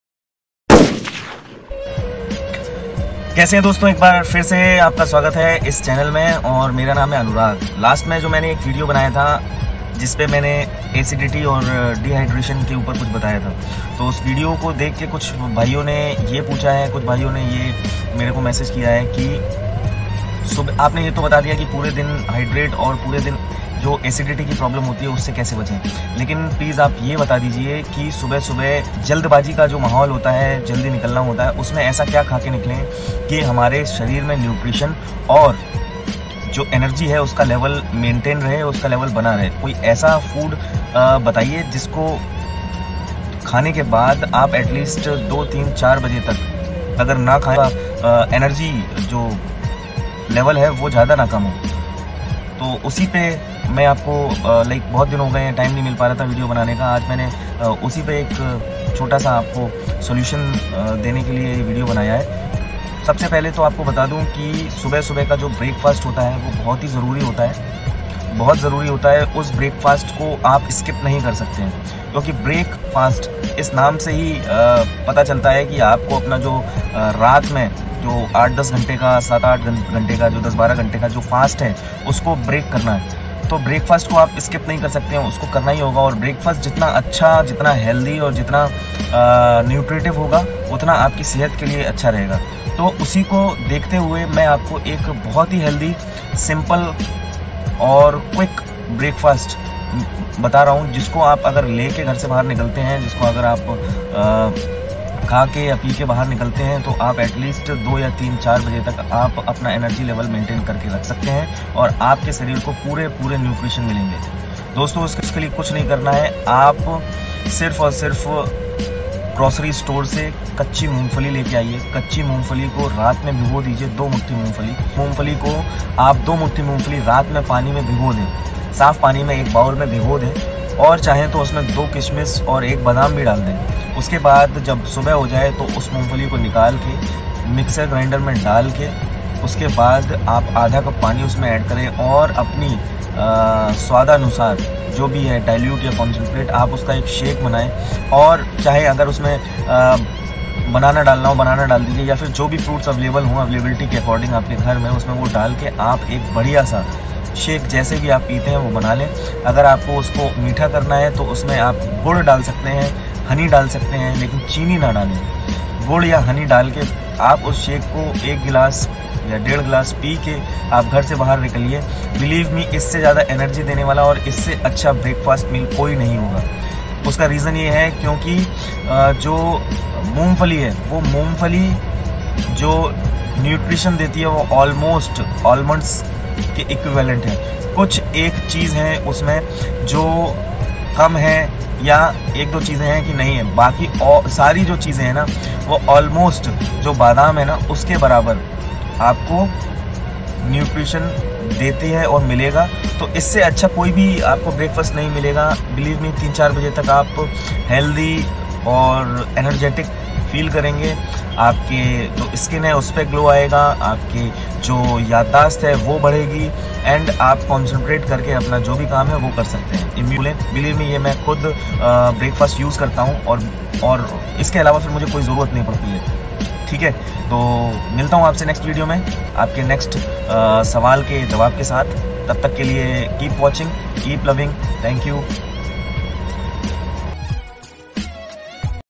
Best Breakfast
In this video we are talking about best breakfast for morning for sales people.
Whatever are your comments and views please mention in comments.